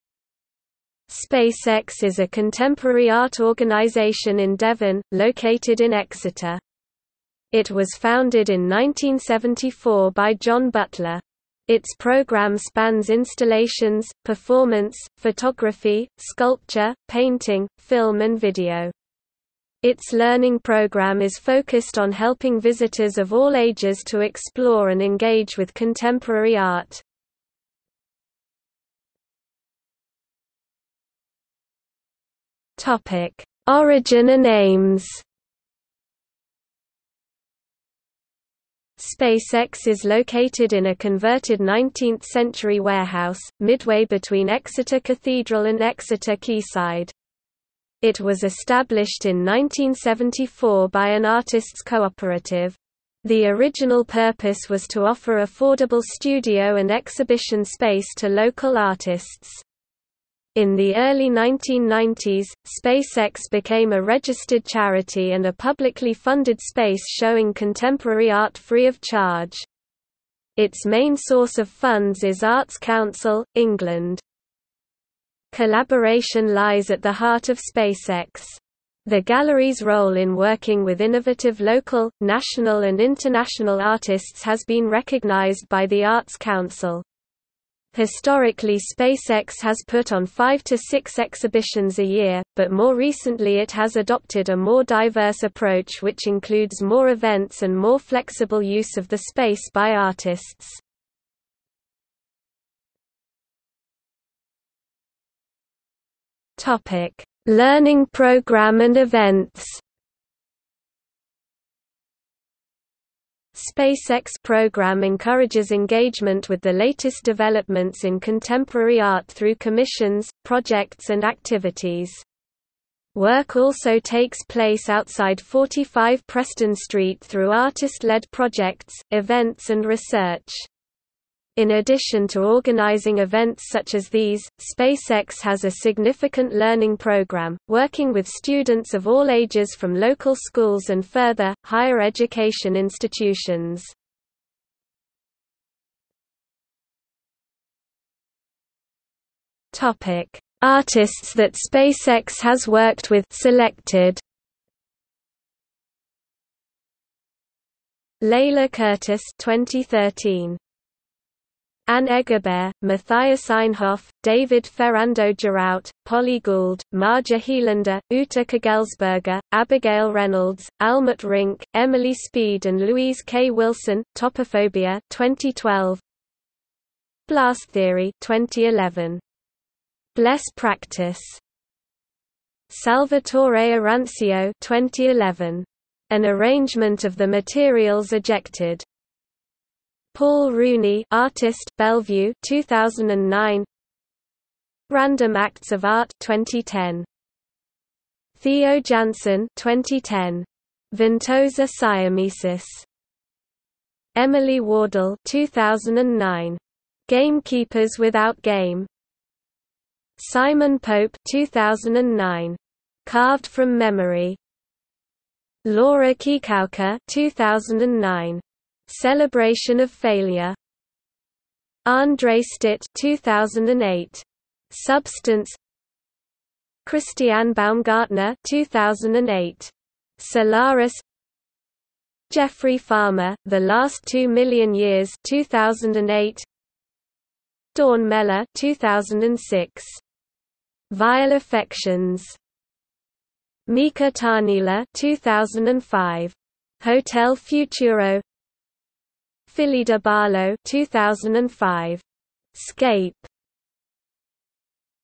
Spacex (art gallery) | Wikipedia audio article
00:00:29 1 Origin and aims
00:01:42 2 Learning programme and events
00:02:26 3 Artists that Spacex has worked with (selected)

- Socrates

SUMMARY
Spacex is a contemporary art organisation in Devon, located in Exeter. It was founded in 1974 by John Butler. Its programme spans installations, performance, photography, sculpture, painting, film and video. It's learning programme is focused on helping visitors of all ages to explore and engage with contemporary art.